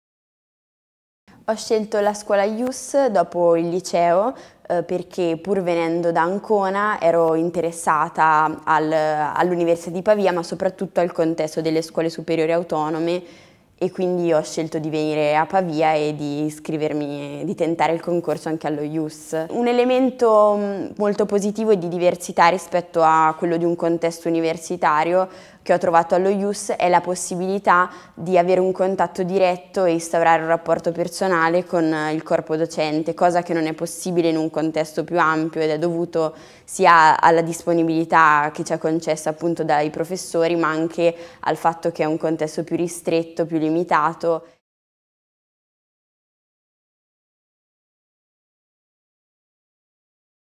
Accountability Report 2016 - La Scuola IUSS vista dagli allievi: Sena Santini
“Un elemento di diversità è la possibilità di instaurare un rapporto personale con i docenti”: l’esperienza di Sena Santini, allieva dei Corsi ordinari della Scuola IUSS Pavia.